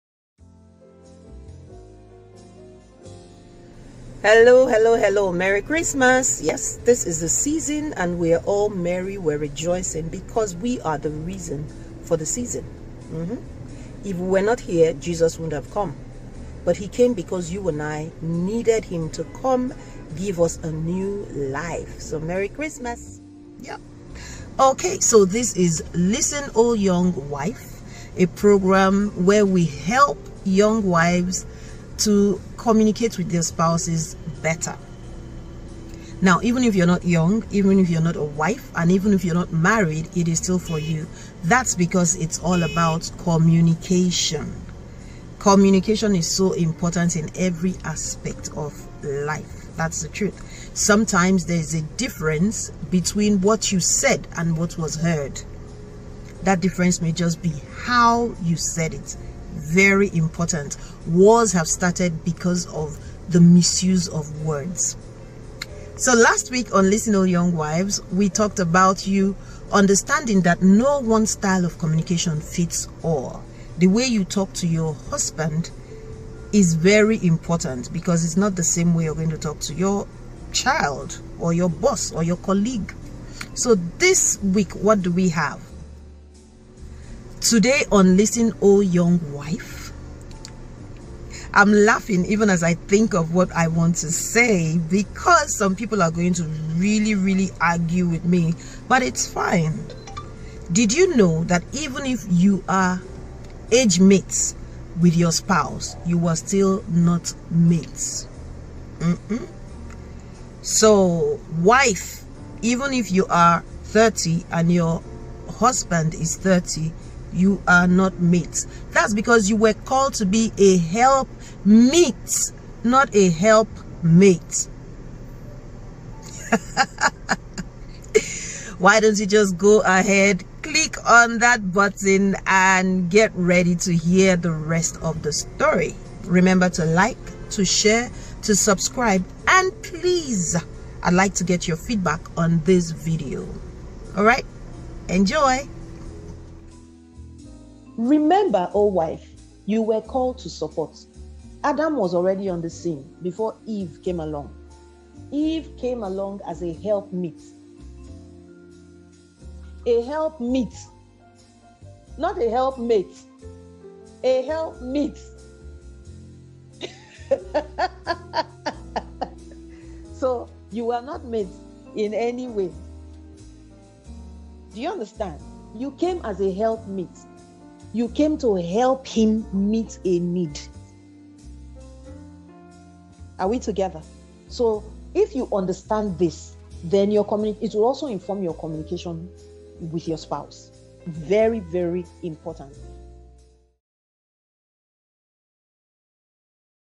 Age Mate or Help Meet??? Listen oh Young Wife #6
Some wives make the mistake of assuming that since they are same age as their spouses, then that suggests that they are mates. certainly not! God sent you oh wife, as a help meet, not age mates. When you discover your true identity as a wife, communication becomes easy...